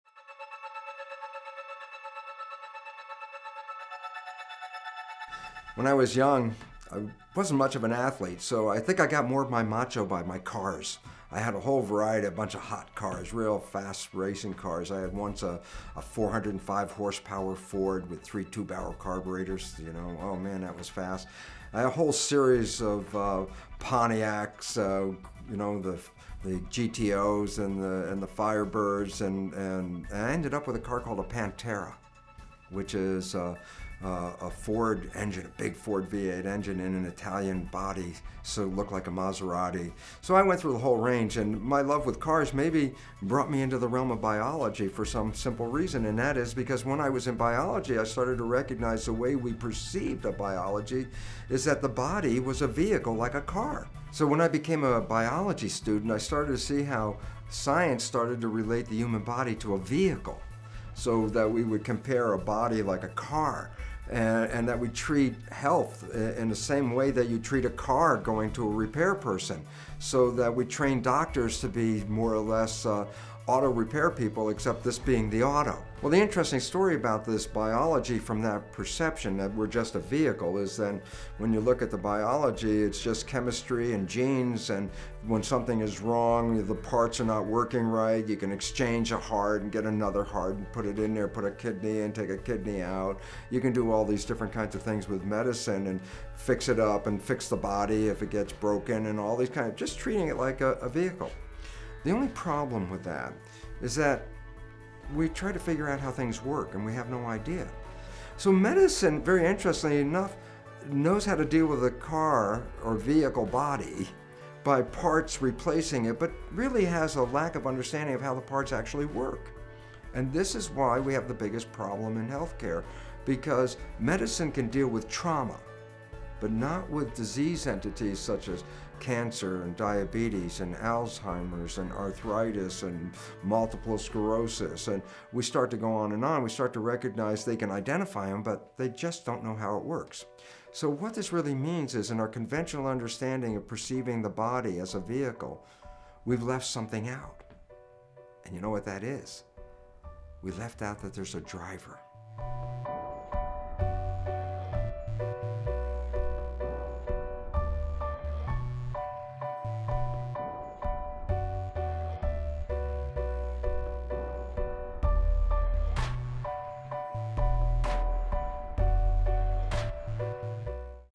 Bruce Lipton - Cars and Biology
The human body is very much like an automobile.  What is the difference between a 'hot headed' driver, and a more responsible driver's effect on the car?  Dr. Bruce H. Lipton gives us a wonderful analogy about the importance of taking care of your body/ vehicle to prevent mechanical/biological damage to your cells.